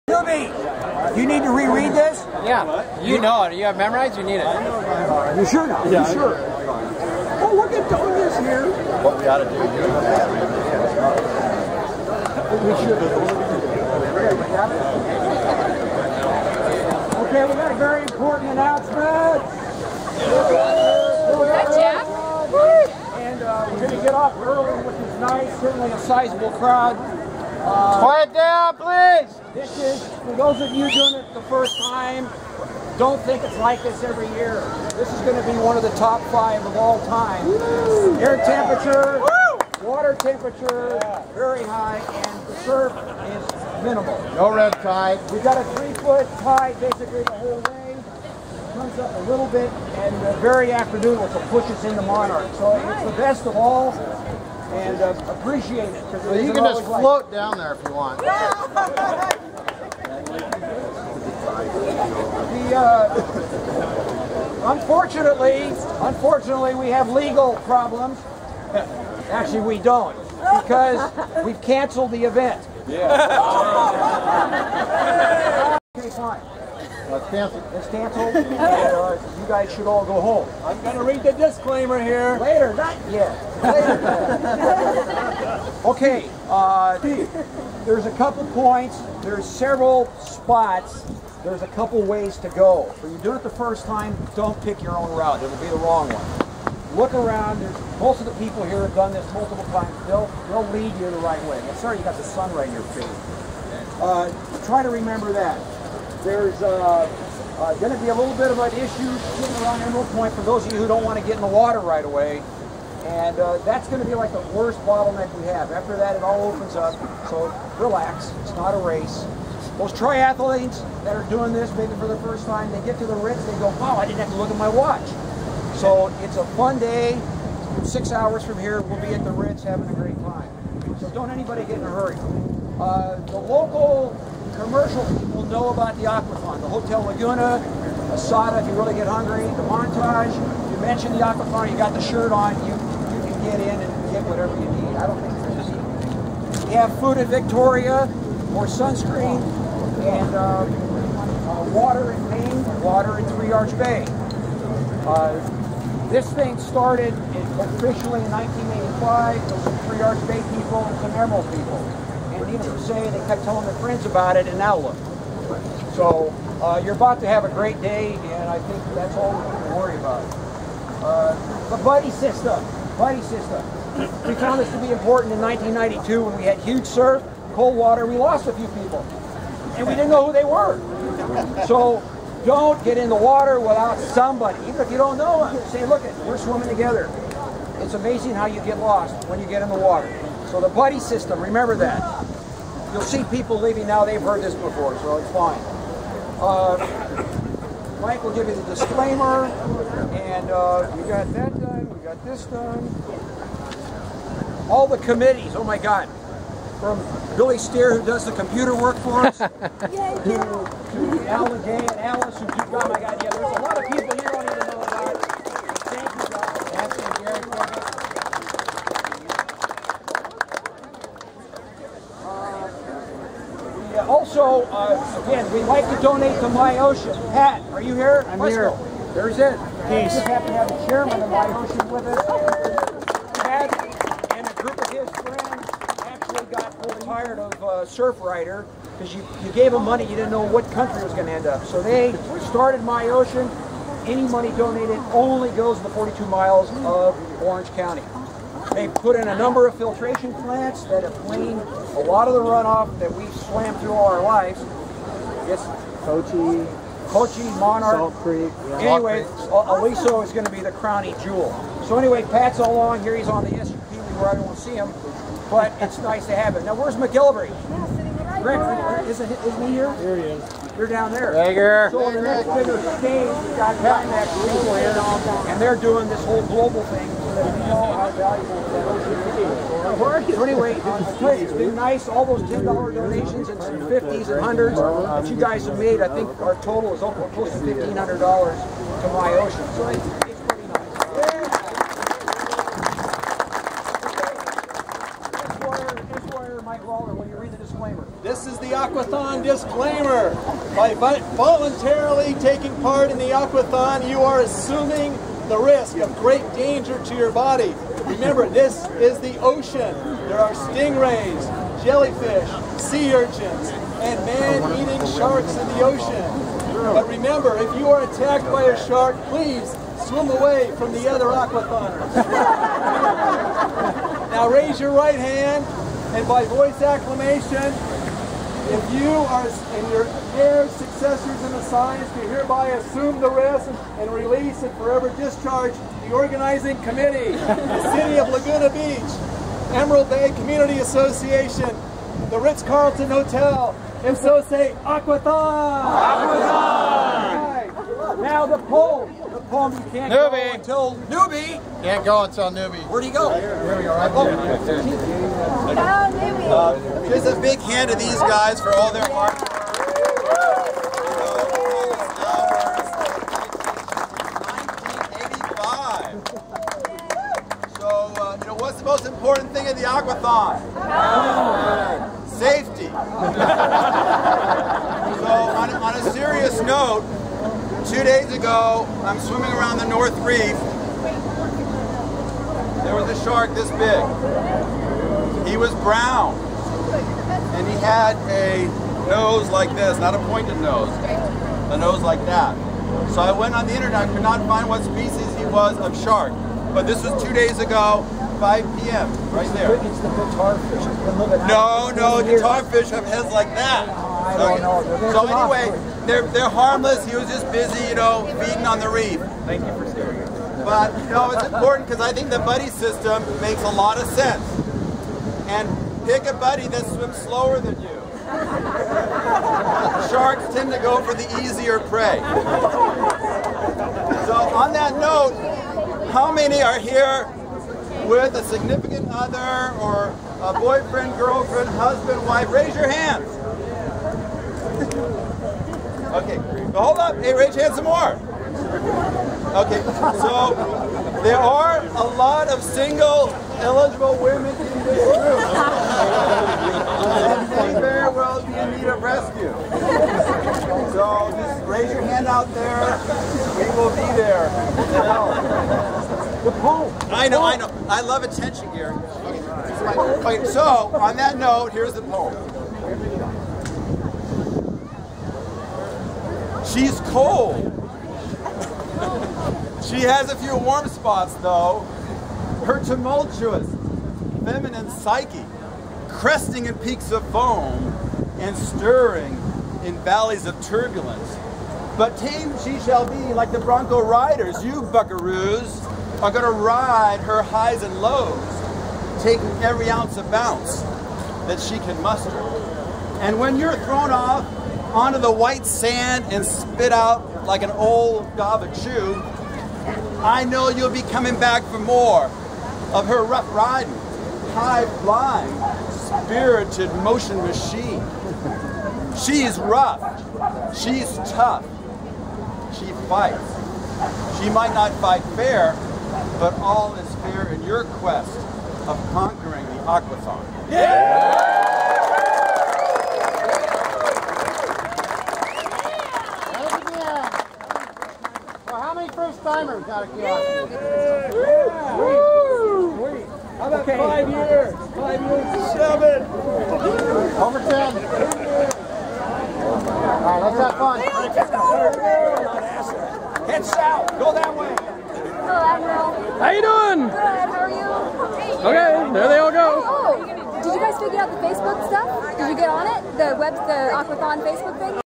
2012 Laguna Aquathon Preamble
floating & walking down the Laguna Beach coast 9 miles then a drink at the ritz